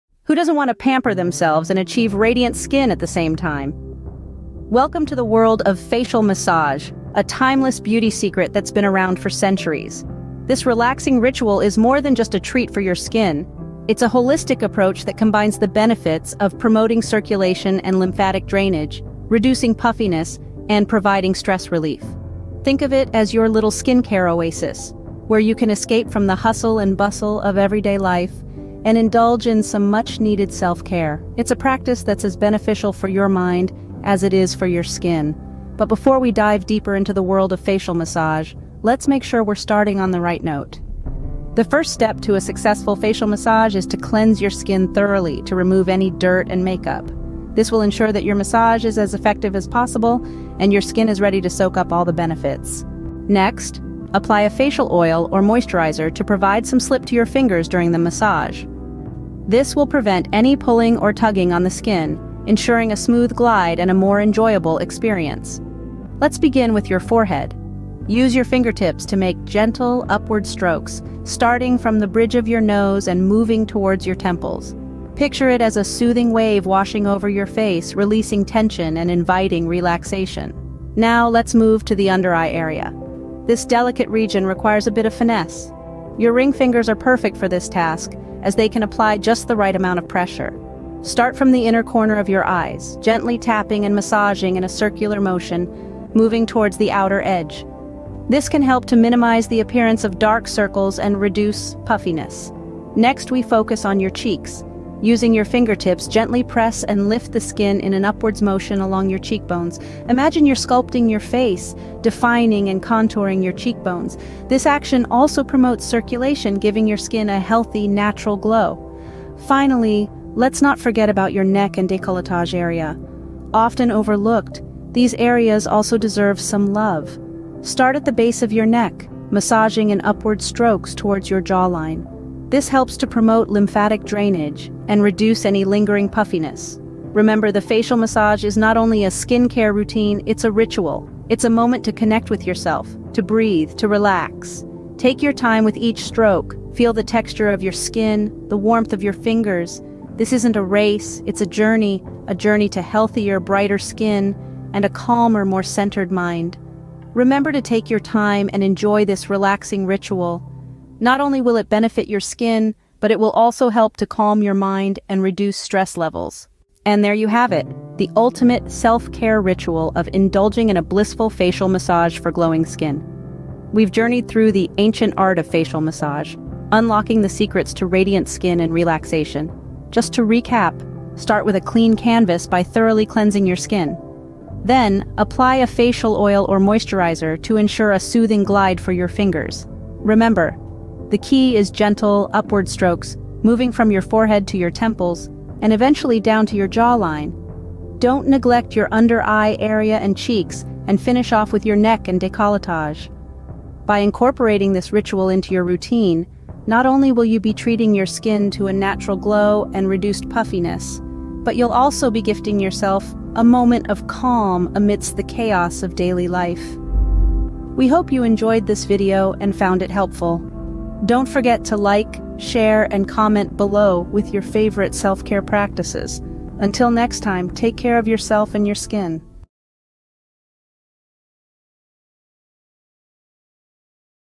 The Ultimate Self-Care Ritual: Indulge in a Blissful Facial Massage for Glowing Skin
Today, we're diving into the ultimate self-care ritual – indulging in a blissful facial massage for glowing skin. Who doesn't want to pamper themselves and achieve radiant skin at the same time? Well, we've got you covered with this relaxing and rejuvenating practice.

Did you know that facial massages have been used for centuries as a way to promote circulation and lymphatic drainage in the skin? This not only helps to reduce puffiness and promote a natural glow but also aids in relaxation and stress relief.

To start your facial massage, make sure to cleanse your skin thoroughly to remove any dirt and makeup. This will ensure that your massage is as effective as possible.

Next, apply a facial oil or moisturizer to provide some slip to your fingers during the massage. This will prevent any pulling or tugging on the skin and make the experience more enjoyable.

Begin with gentle, upward strokes on your forehead, moving towards your temples, and then down to your jawline. This will help to release tension and promote relaxation in the facial muscles.

Don't forget to pay special attention to your under-eye area by using your ring fingers to gently tap and massage in a circular motion. This can help to reduce dark circles and puffiness.

Moving on to your cheeks, use your fingertips to gently press and lift the skin in an upwards motion. This will help to define and contour your cheekbones while also promoting circulation.

Finally, finish off your facial massage by gently massaging your neck and décolletage area in upward strokes to promote lymphatic drainage and reduce puffiness.

Remember to take your time and enjoy this relaxing ritual. Not only will it benefit your skin, but it will also help to calm your mind and reduce stress levels.

And there you have it – the ultimate self-care ritual of indulging in a blissful facial massage for glowing skin. We hope you enjoyed this video and found it helpful. Until next time, take care of yourself and your skin.